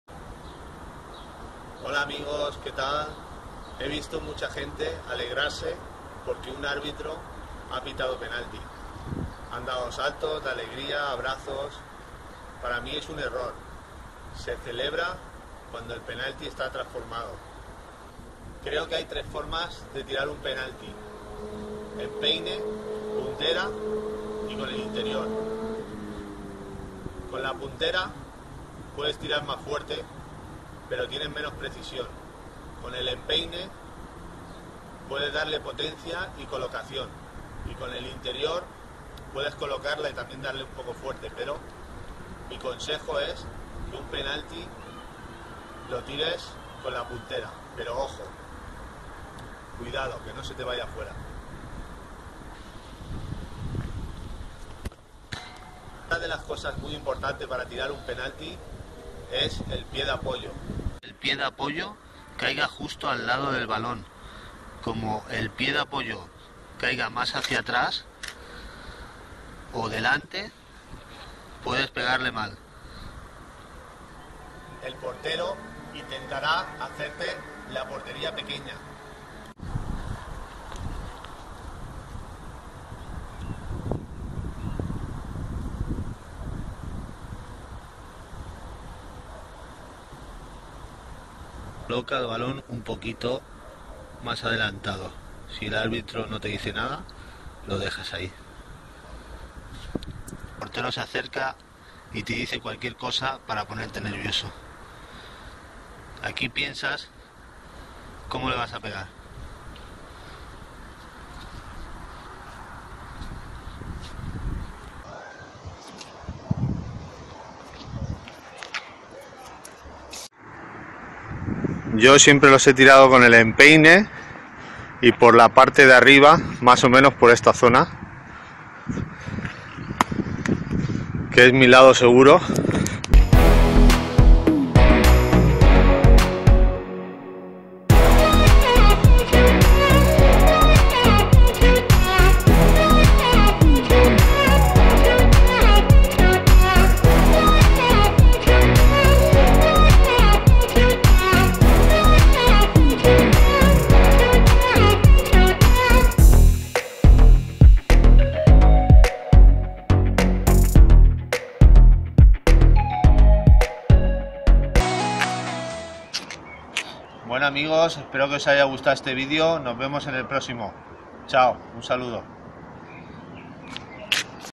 "Como tirar un penalti en futsal"
Un video tutorial de como tirar un penalti en fútbol sala.En futbol sala puedes tirar un penalti hasta de rabona pero..las tres formas mas seguras son:empeine,puntera o interior..Mi forma preferida siempre a sido el empeine,arriba a un lado y fuerte.Pero si no lo tienes claro,suelta un punterazo fuerte y que valla a puerta!!